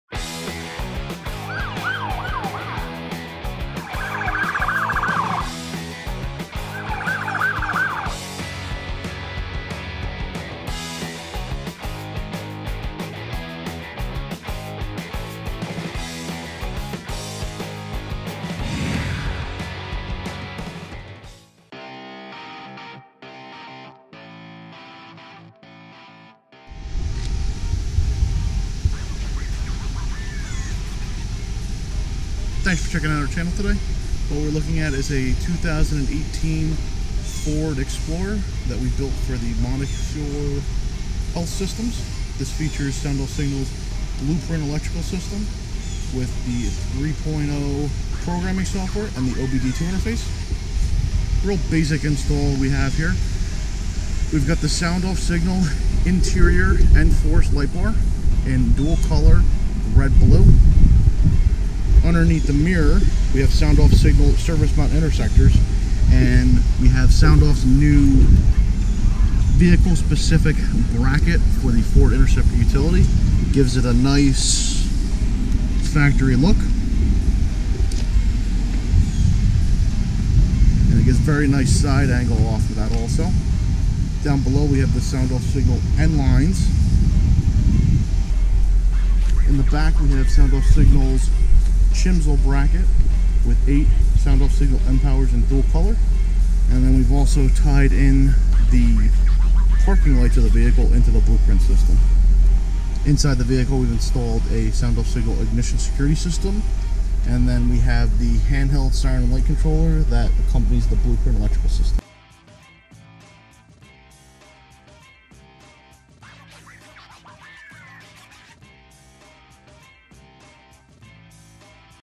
Montefiore Health Systems 2018 Ford Explorer Response Vehicle 10-75 Emergency Vehicles
The Montefiore Health Systems worked with us to design and manufacture their new response vehicle.